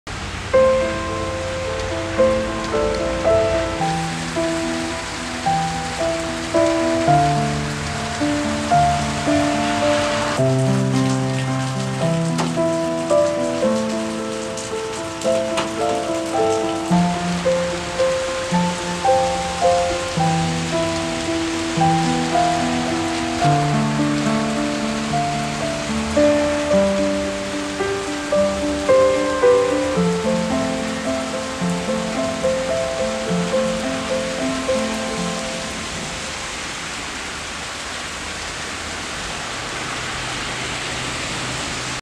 Autumn Rain
I made this as a quick test for the Rokinon 50mm Cine lens in low-light conditions. I was pleasantly surprised with the quality of the footage I was able to obtain with a Canon Rebel t6. Unfortunately, as my heavy tripod was in the car (and I don't have a garage), some of the motions aren't as fluid as I would like.